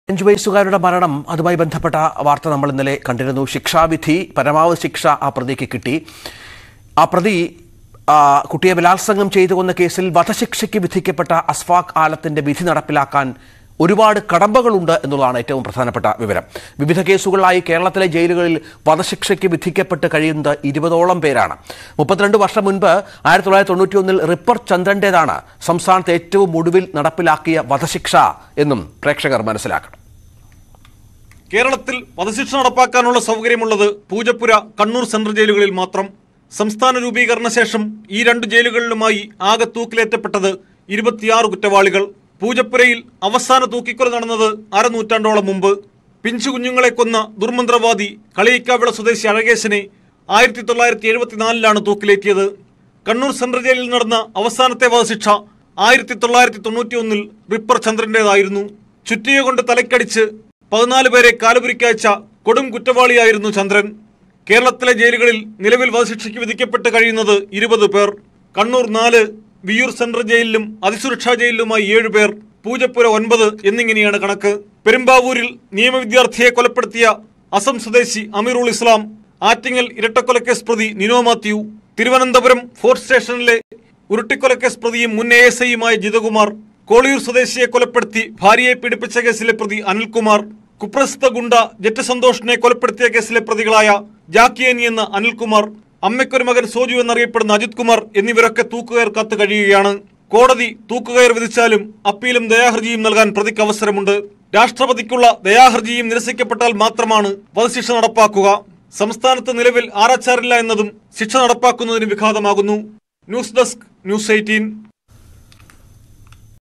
Aluva Child Case | സംസ്ഥാനത്ത് ആരാച്ചാർ ഇല്ല ! Asfaq Alam ന്റെ വിധി മാറുമോ?
Aluva Child Case :ആലുവയിലെ അഞ്ചു വയസ്സുകാരിയെ ബലാത്സംഗം ചെയ്തു കൊന്ന കേസിൽ വധശിക്ഷയ്ക്ക് വിധിക്കപ്പെട്ട Asfaq Alamന്റെ വിധി നടപ്പാക്കാൻ കടമ്പകളേറെ. വിവിധ കേസുകളിലായി കേരളത്തിലെ ജയിലുകളിൽ വധശിക്ഷയ്ക്ക് വിധിക്കപ്പെട്ടു കഴിയുന്നത് 20 പേരാണ്. 32 വർഷംമുമ്പ് 1991ൽ റിപ്പർ ചന്ദ്രന്റേതാണ് സംസ്ഥാനത്ത് ഏറ്റവുമൊടുവിൽ നടപ്പാക്കിയ വധശിക്ഷ.